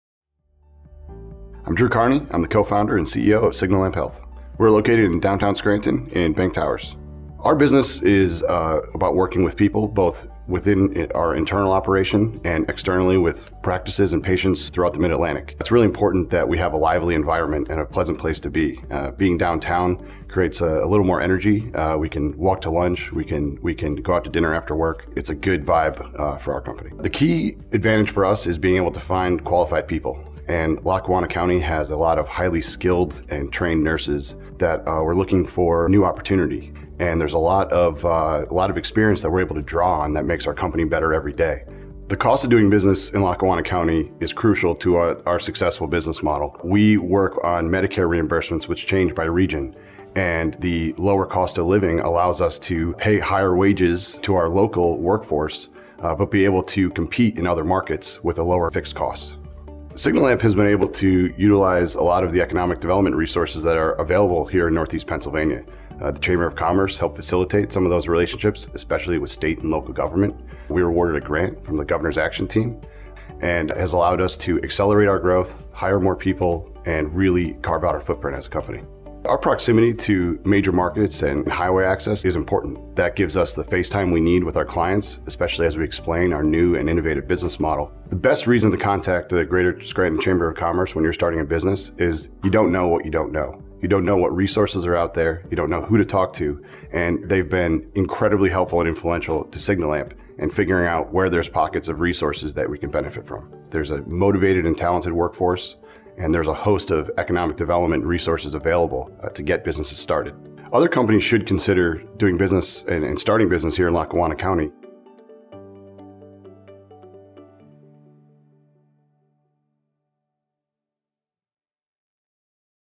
Powered By Lackawanna: Signallamp Health
Signallamp Health tells about the advantages of being located in Lackawanna County, including a skilled workforce and the Chamber's economic development resources.